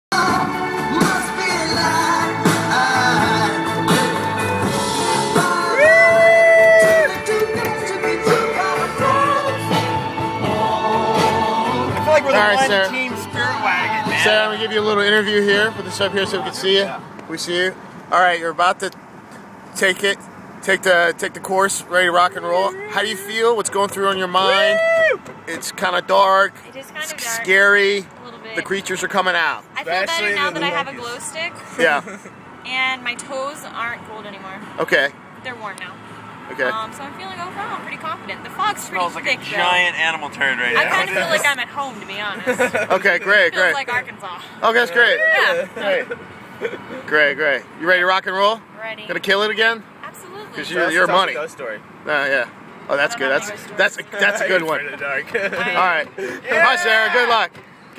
Ragnar DC 2011 Team Don't Sweat the Petty Things; Don't Pet the Sweaty Things
Ragnar Relay 2011, Washington DC.
Sherwin Interviews Sarah